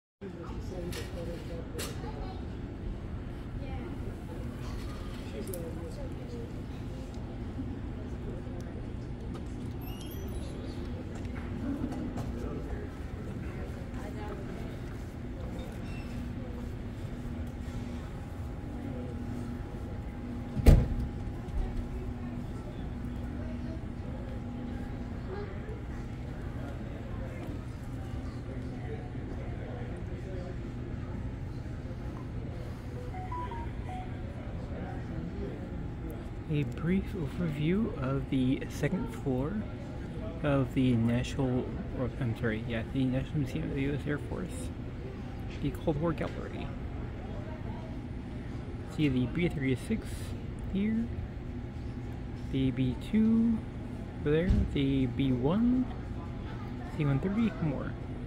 2nd Floor view of the Cold War Gallery at the National Museum of the USAF
2nd Floor overview of the National Museum of the United States Air Force in the Cold War Gallery